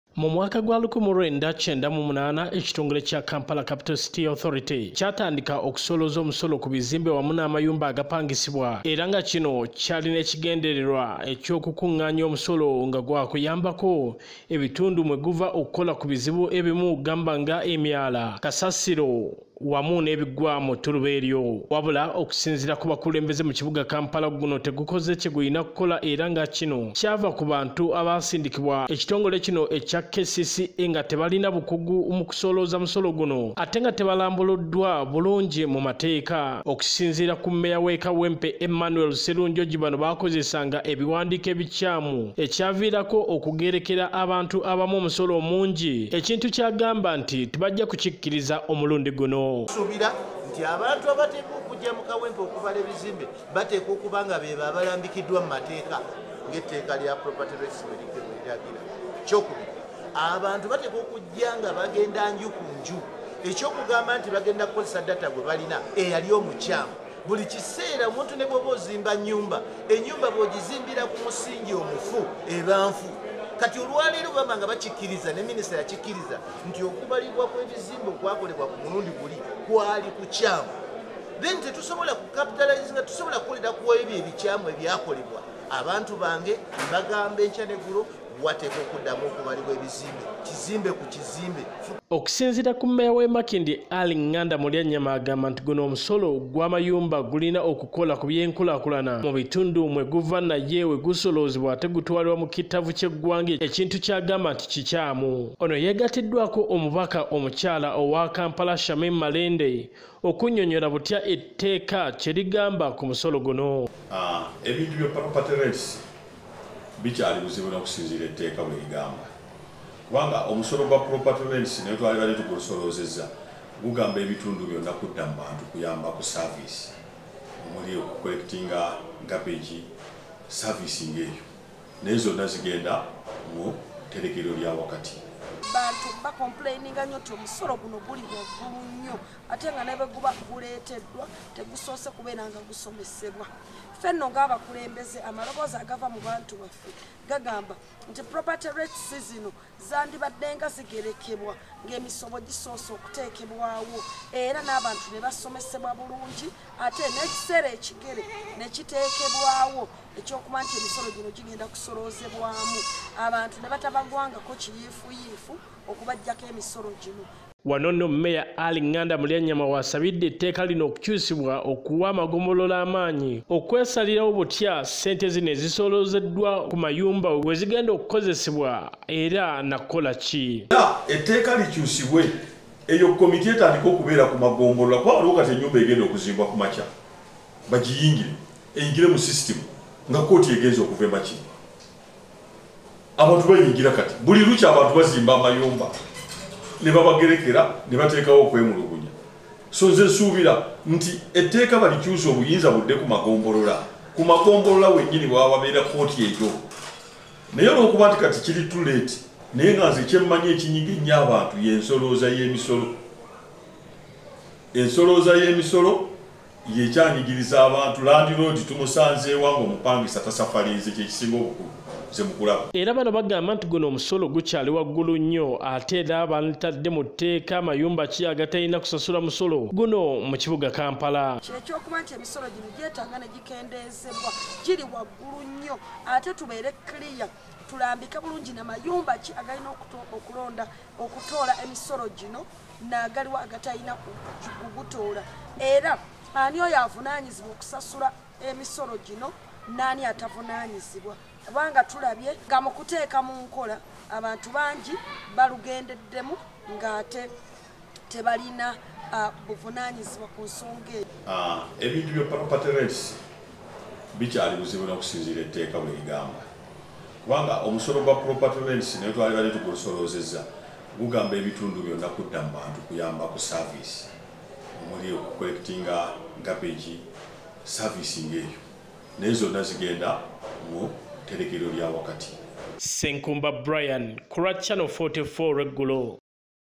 PROPERTY TAX TO HIGH KAMPALA LEADERS
LOWER ON THE PROPERTY TAXES IN KAMPALA LEADERS TASK GOVERNMENT